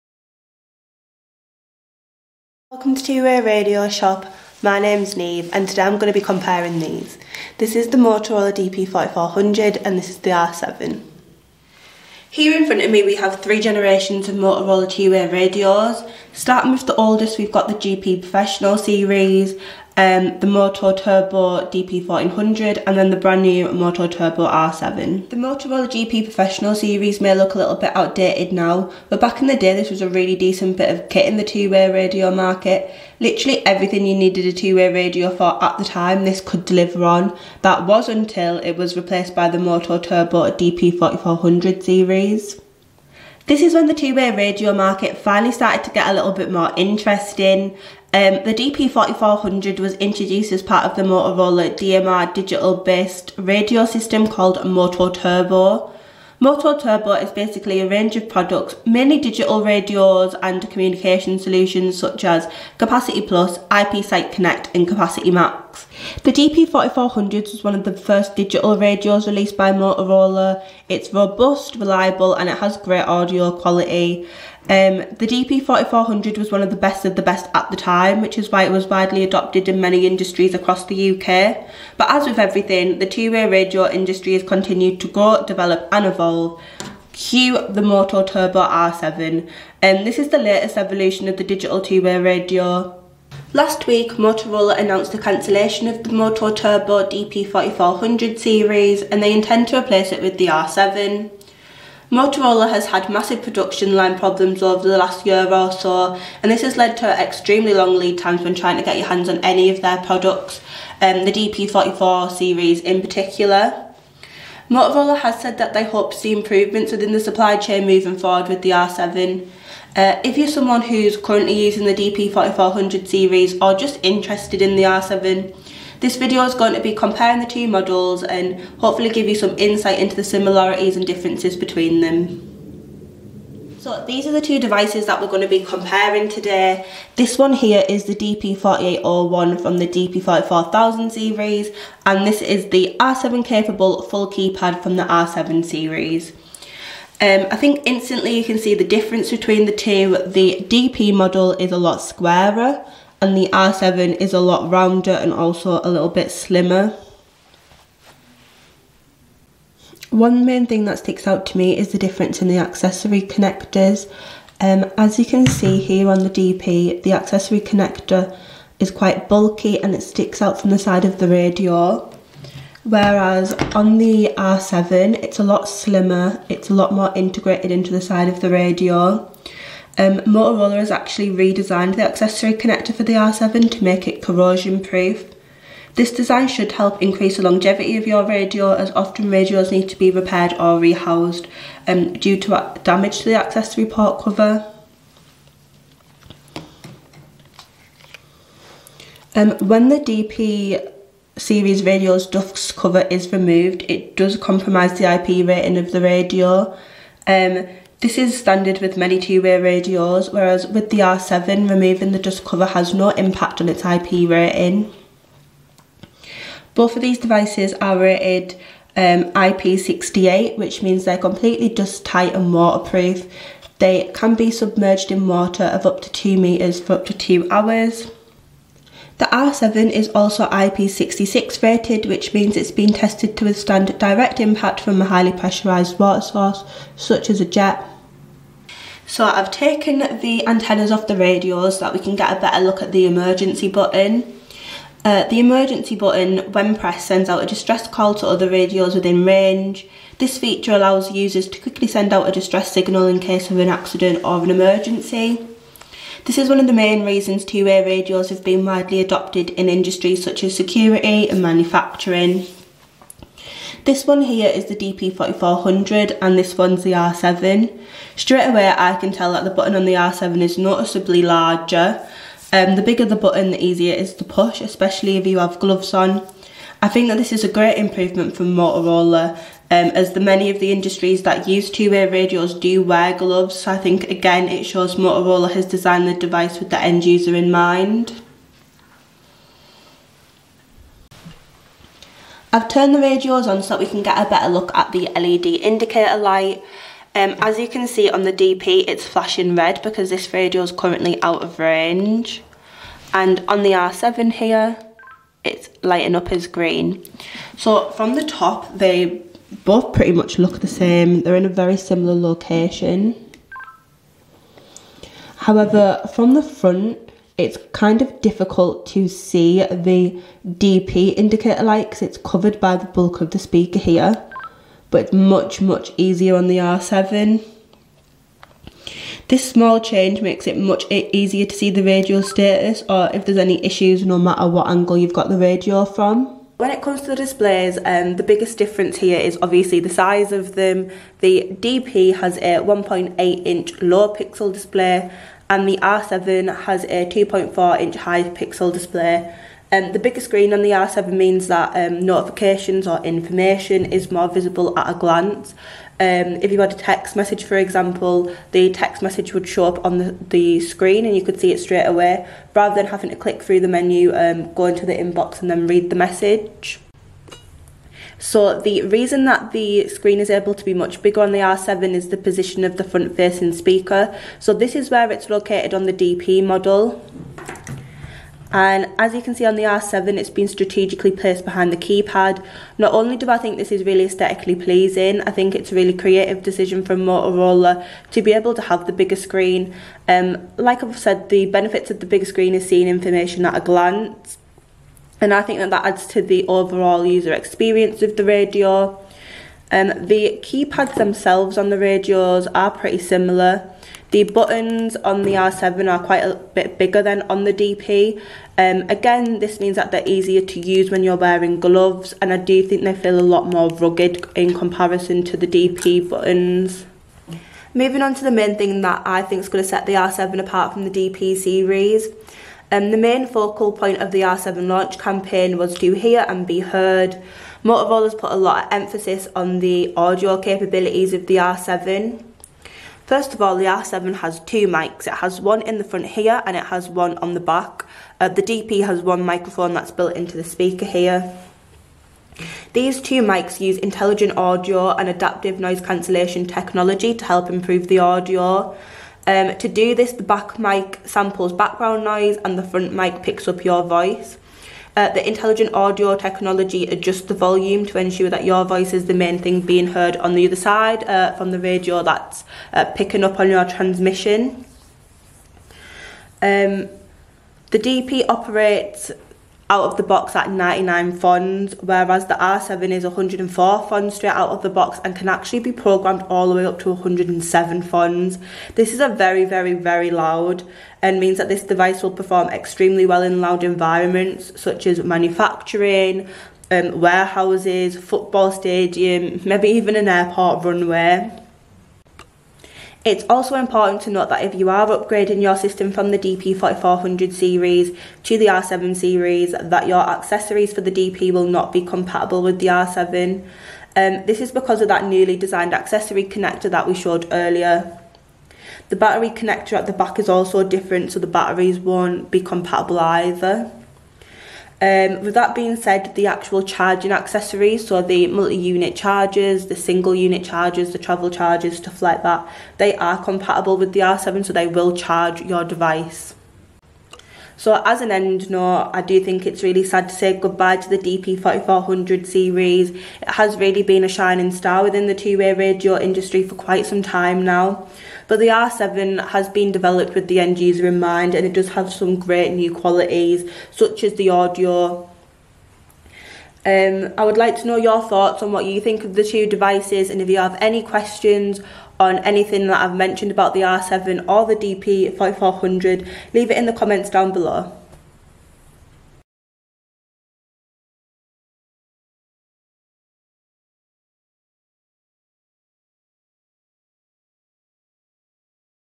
NEW Motorola R7 & DP4000e Series : Comparison (2023)
Earlier last week Motorola announced the cancellation of the MOTOTRBO DP4000e series of two-way radios and plan to replace it with the MOTOTRBO R7.

In this video, we review the similarities, differences and improvements between the two Motorola models!

All 3 variations of the New MOTOTRBO R7 are available to buy on our